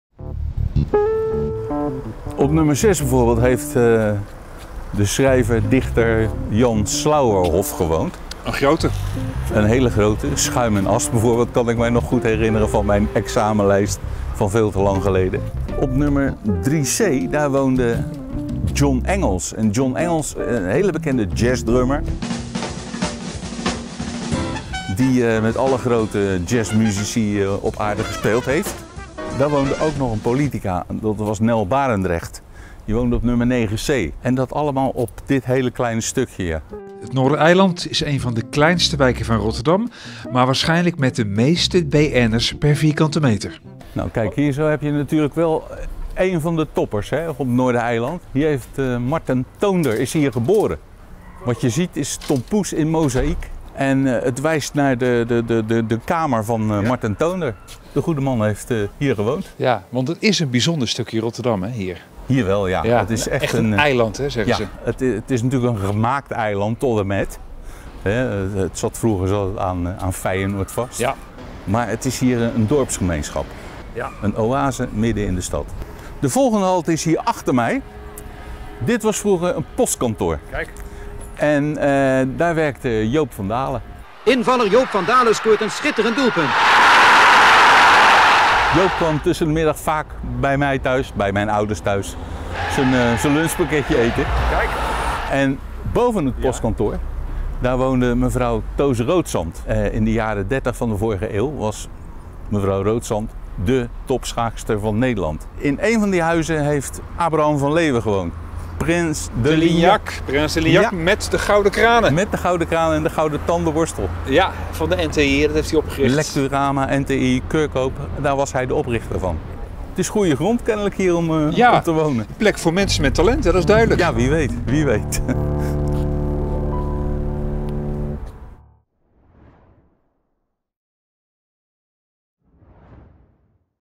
Hier woonden de Bekende Rotterdammers op het Noordereiland
Schrijver Jan Slauerhoff, stripauteur Marten Toonder en voetballer Joop van Daele: zomaar een greep in de namen van bekende Rotterdammers die op het Noordereiland te vinden waren. Het is één van de kleinste wijken van Rotterdam, maar bijna nergens in de stad vind je waarschijnlijk zoveel beroemde personen per vierkante meter.Historicus Peter Bakker geeft een rondleiding over het eiland. Hij kijkt naar een pand met Tom Poes van mozaïek op het dak. Daar is Toonder geboren. Het beeld van Tom Poes wijst naar de kamer waar de overleden striptekenaar is geboren. &quot;Het Noordereiland is een gemaakt eiland, want vroeger zat het aan de wijk Feijenoord vast&quot;, vertelt Bakker. &quot;Maar het is hier een dorpsgemeenschap: een oase in de stad.&quot;Zo bewees ook oud-voetballer Van Daele, die werkte op het voormalige postkantoor op het Noordereiland. &quot;Tussen de middag kwam hij vaak bij mijn ouders thuis zijn lunchpakketje eten&quot;, herinnert Bakker zich nog. Boven het oude postkantoor woonde ook nog eens een bekende Rotterdamse: Toos Roodzant, de beste schaakster van Nederland. &quot;Het is goede grond om hier te wonen&quot;, knipoogt Bakker. BombardementEr is een diepere reden voor het grote aantal beroemdheden wellicht. &quot;Na het bombardement op Rotterdam was er woningnood in het centrum. Veel bewoners verhuisden naar het Noordereiland.&quot; Dat nieuwe bloed deed wat met de buurt.Naast genoemde namen woonde ook Bram van Leeuwen, ook wel bekend als de Prince de Lignac op het eiland. Van Leeuwen vergaarde een fortuin met ondermeer het Nederlands Talen Instituut (NTI) en liet de guldens goed rollen. Zo bezat hij een jacht met gouden kranen. Logés gingen steevast naar huis met een gouden tandenborstel.Peter Bakker is altijd nieuwsgierig naar nog meer bekende &apos;eilanders&apos;.